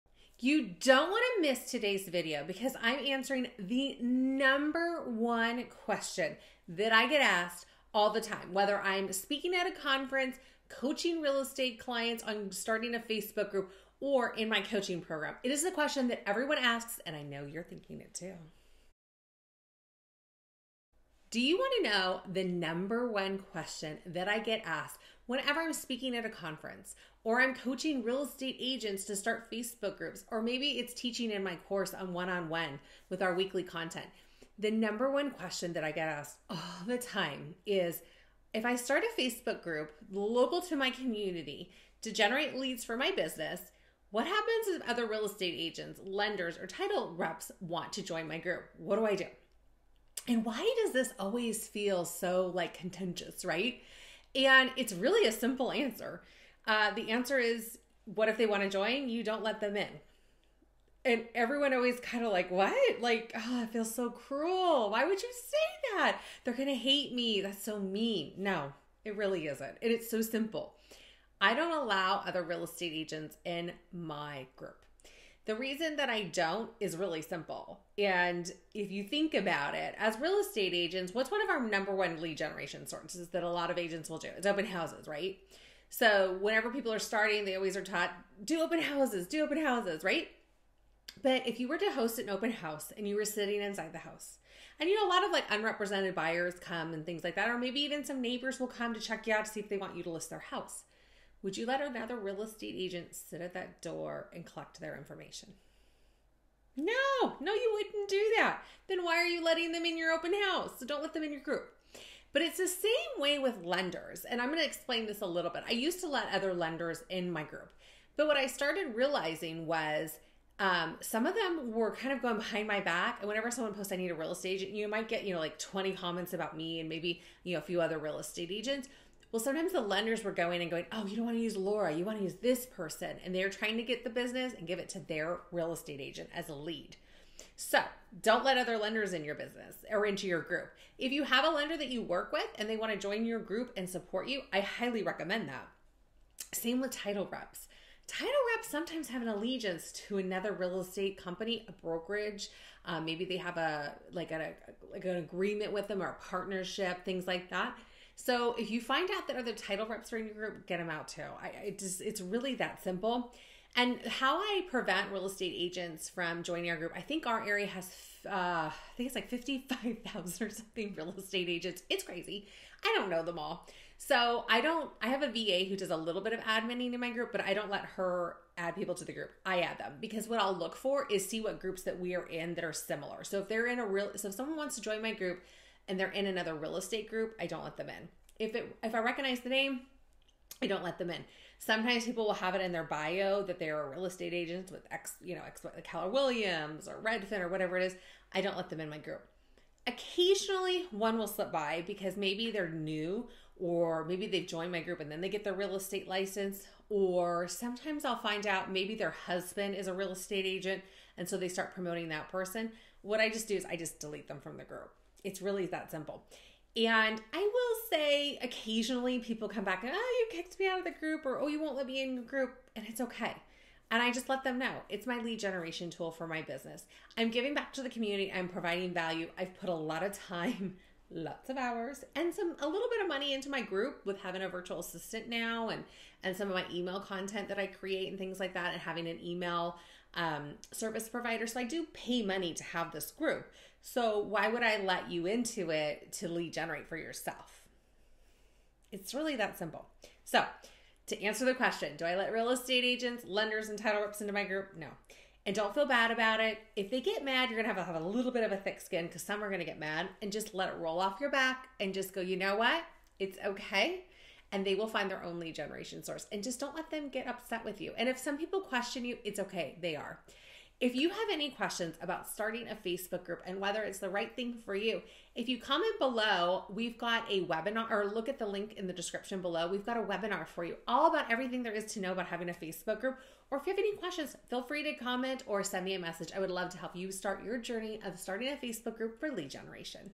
Should You Let Competitors into Your Real Estate Facebook Group? | Top Agent Secrets
🔑 **Discover the Answer to the #1 Question for Real Estate Facebook Groups!**

Are you a real estate agent looking to grow a thriving Facebook community but stuck on one crucial question: Should you allow other agents, lenders, or title reps into your group? It’s a common dilemma, and today, I’m diving deep into this hot topic to give you clarity and control over your Facebook Group strategy!

🏡 **What’s Inside:**
- ✅ **Pros & Cons:** I’ll walk you through the advantages and disadvantages of having industry peers in your group.
- 🎯 **Strategy Overviews:** Understand how to leverage the presence of other professionals for your group’s and your business's benefit.
- 🤝 **Community Building Techniques:** Tips on nurturing a supportive environment while keeping your lead generation goals on track.

**Key Takeaways:**
- 🚫 Understand when inviting other professionals might hurt your business.
- 🌱 Learn how collaboration can sometimes lead to unexpected opportunities.
- 📊 Find out how to maintain the perfect balance for maximum group growth.

**Don't Make This Mistake:**
Many agents go one way or the other without considering the strategic implications. Watch this video to make an informed decision that will benefit your business for years to come.

- 📈 For more in-depth coaching and personalized strategies, visit groupstoleads.com